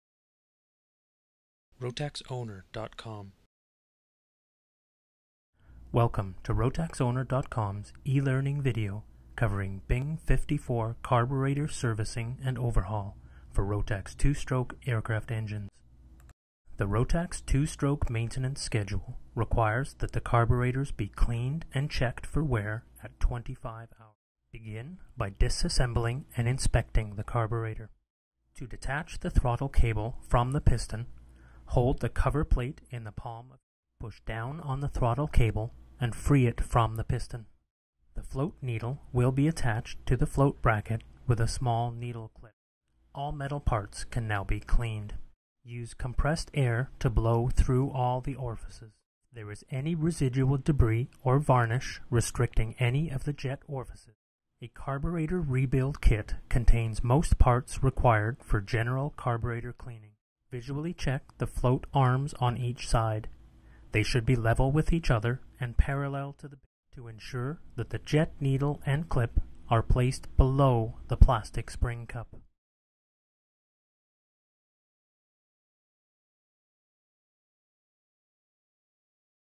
2 Stroke Carburetor Rebuild Bing 54
This e-Learning video covers servicing and overhauling a Bing 54 carburetor on a Rotax 2 stroke aircraft engine.
Watch a complete carburetor tear down, inspection and rebuild.